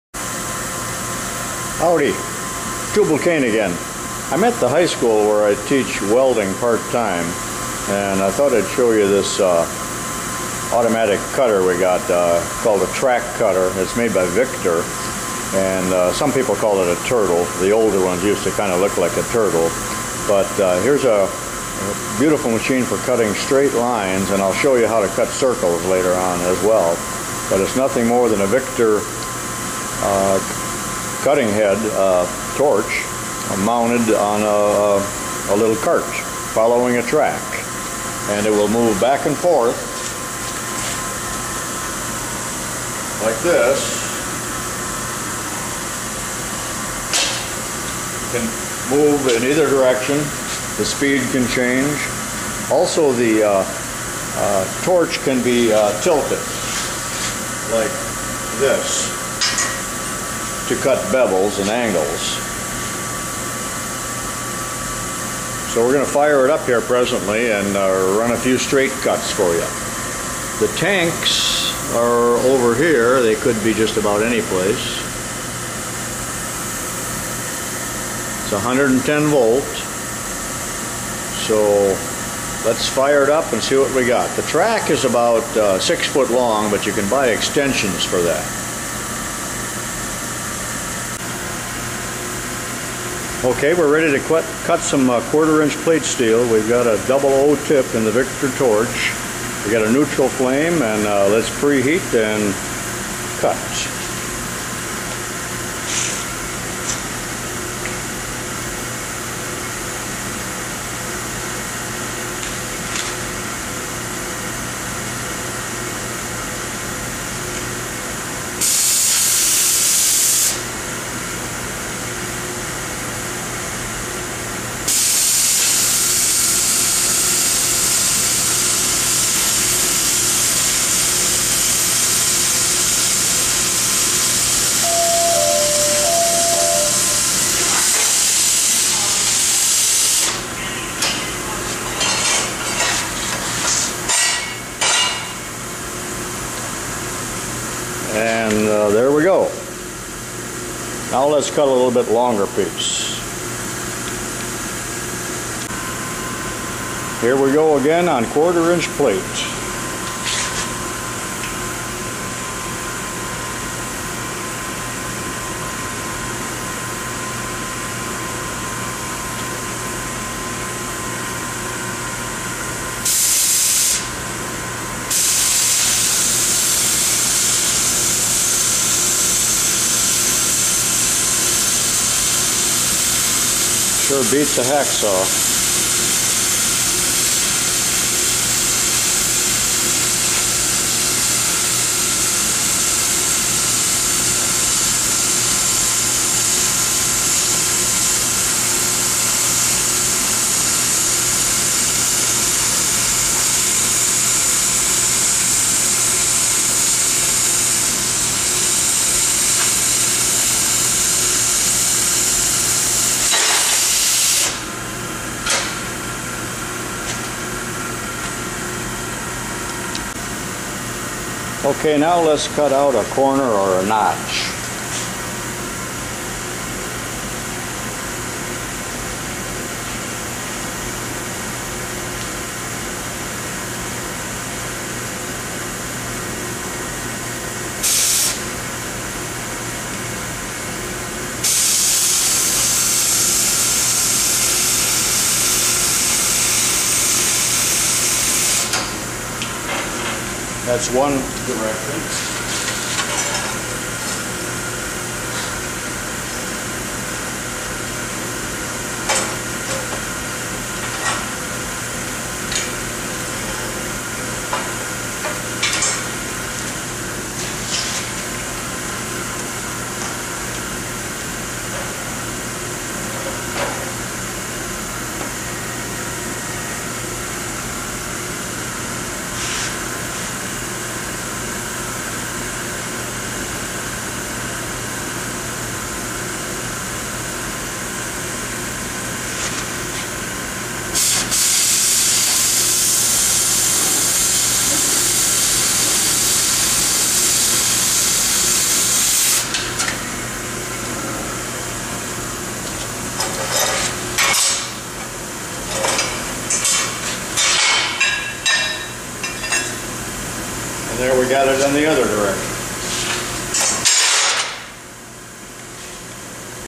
MACHINE SHOP TIPS #38 Flame Cutting Straight Part 1 tubalcain
Tubalcain shows how to flame cut straight lines with the VICTOR track cutter. Oxy-acetylene cutting torch.